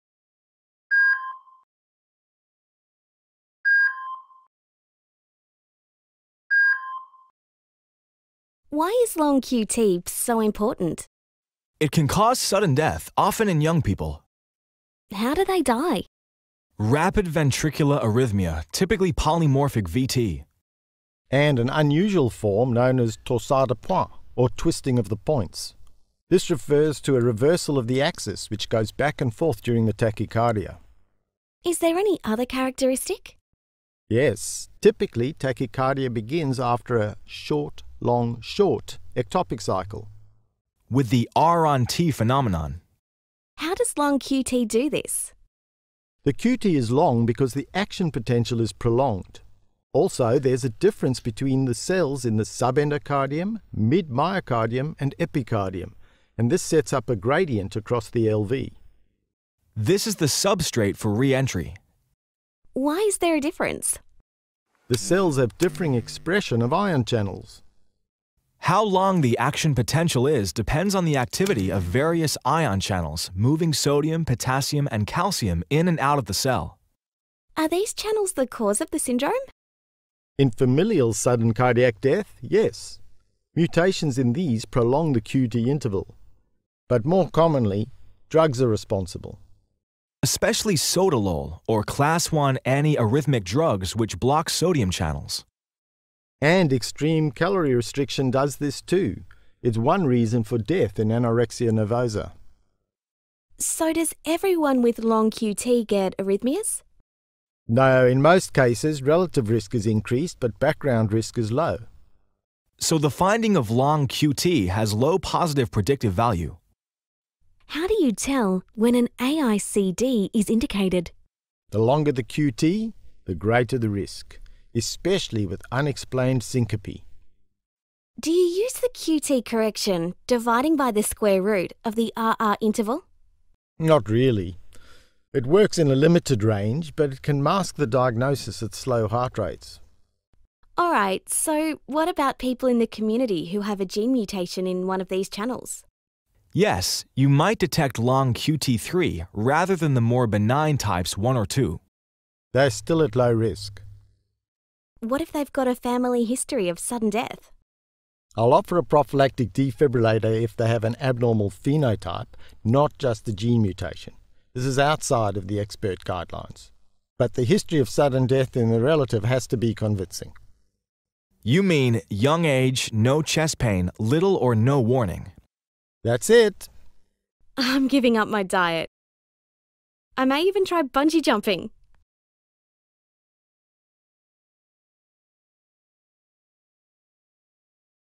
Long QT interval
Ion channels move sodium, potassium and calcium in and out. Not all cells have the same action potential duration.